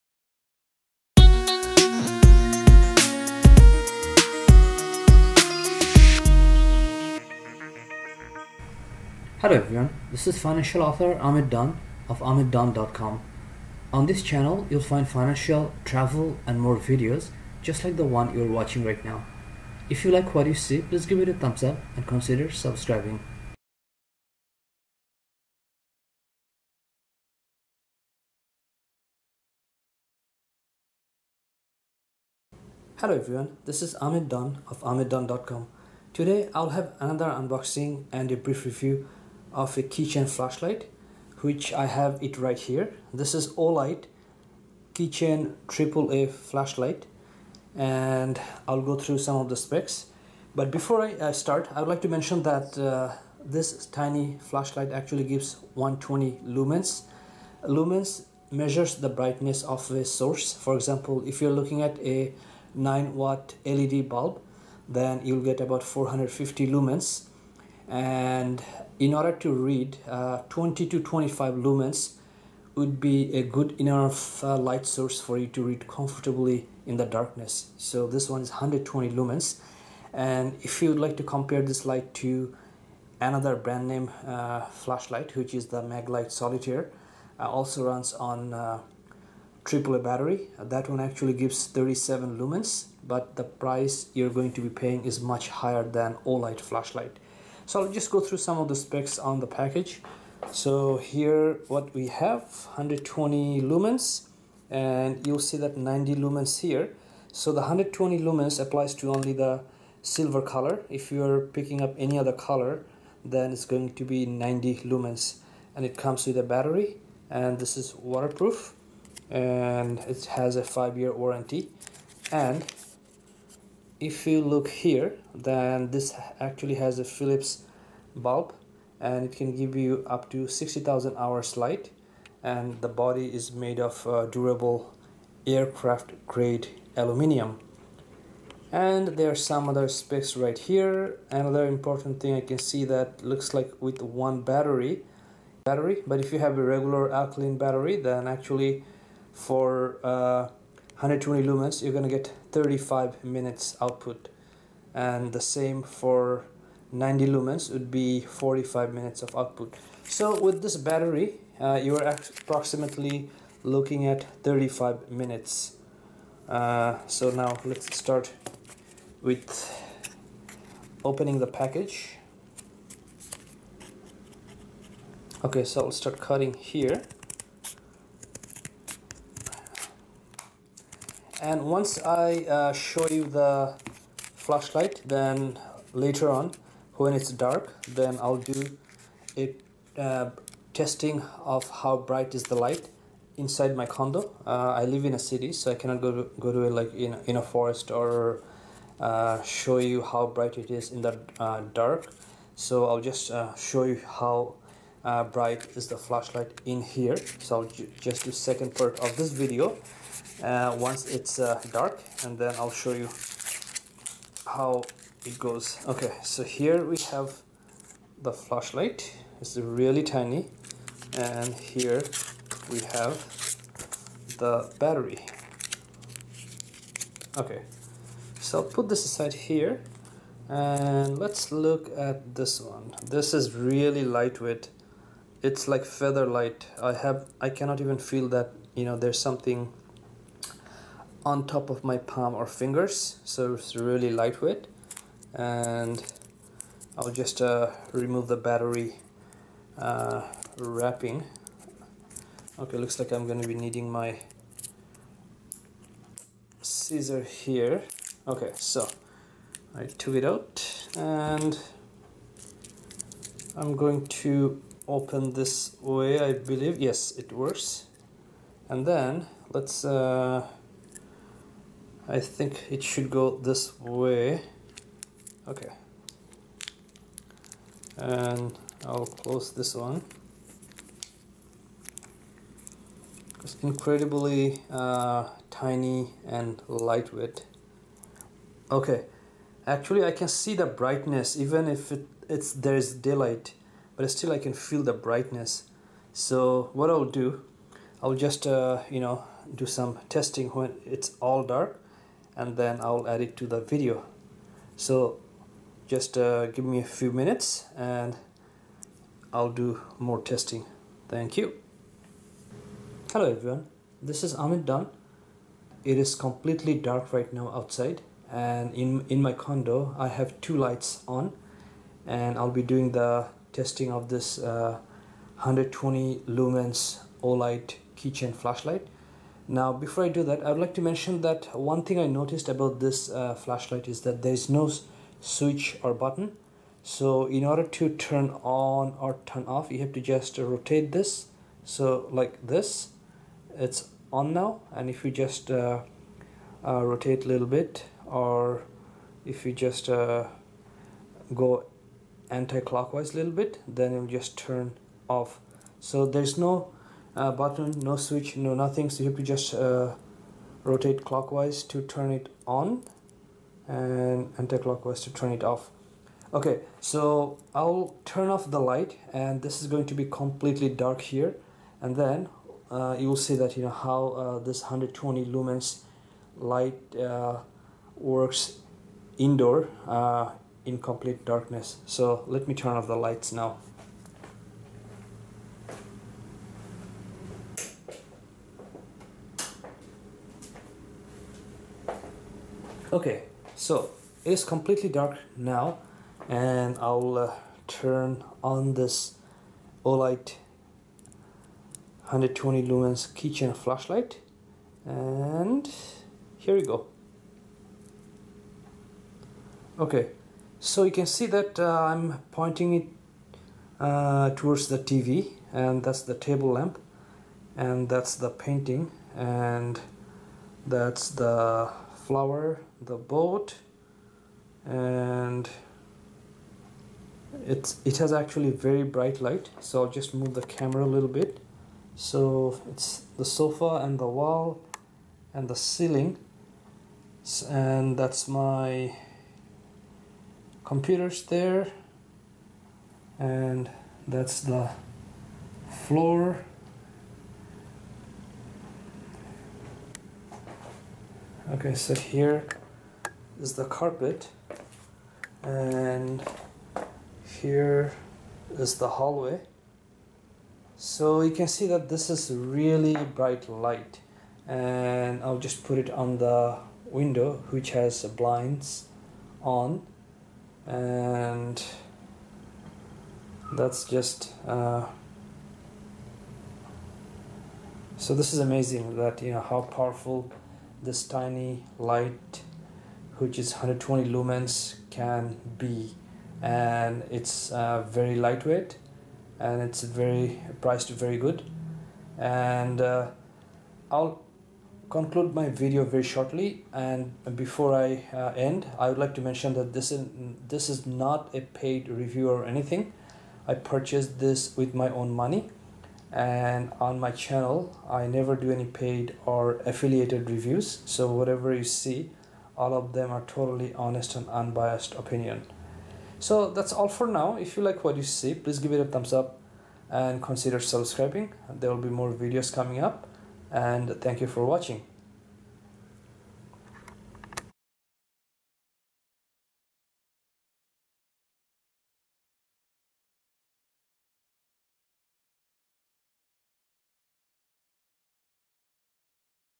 Olight AAA Keychain Flashlight Unboxing, Review, & Indoor Test by Ahmed Dawn
Compact key-ring Pocket Lights  Olight® I3E EOS AAA Keychain Flashlight Mini EDC Torche outputs 120 Lumens (silver only) with Philips LUXEON TX LED. Other colors provide 90 lumens. Body is made of aircraft-grade aluminum with anti-scratch Milspec type-III anodizing. Very light weight and small, but powerful.

In this video I will review and test this handy flashlight in indoor dark environment.